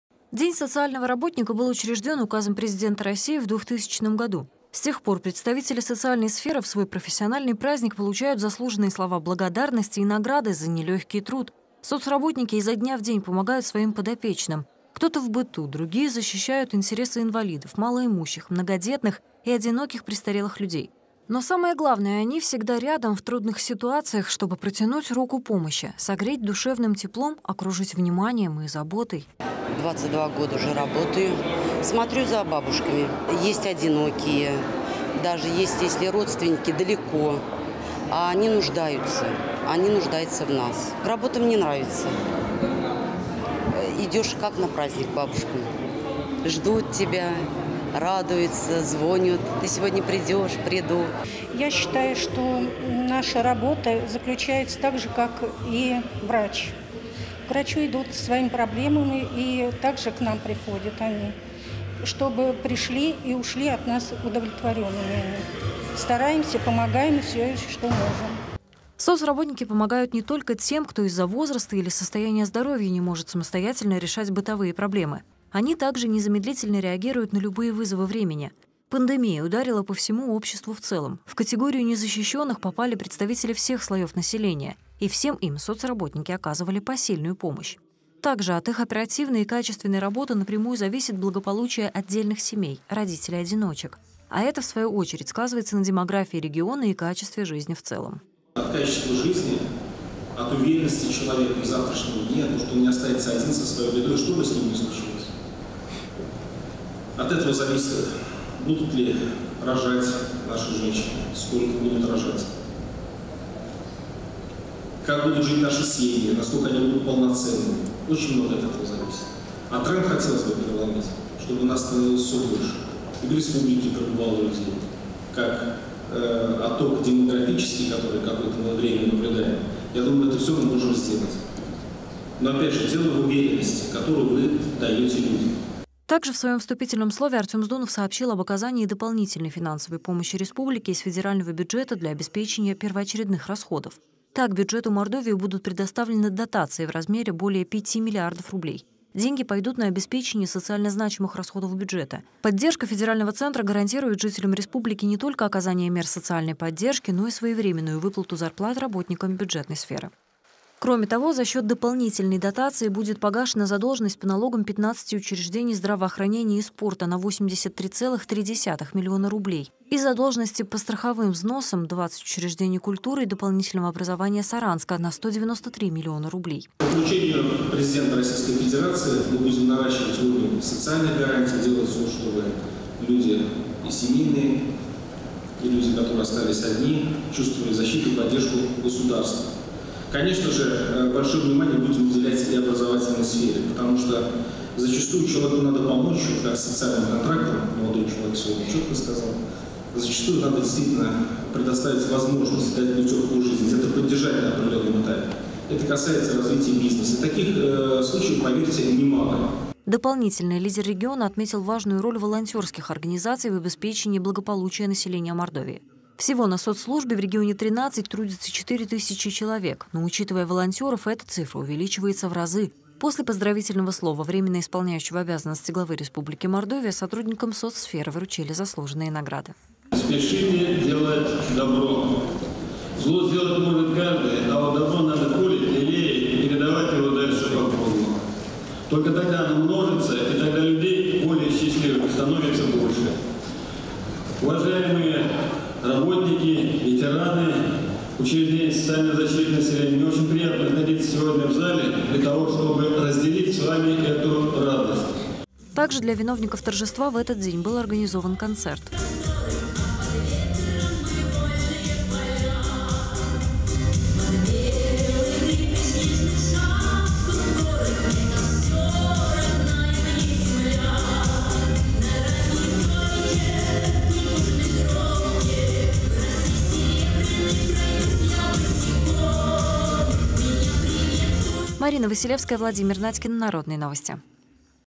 Торжество ко Дню социального работника
8 мая в России отмечают день социального работника. По этому случаю в государственном музыкальном театре имени Яушева состоялось торжественное собрание с участием временно исполняющего обязанности главы Республики Артёма Здунова. Мои коллеги продолжат.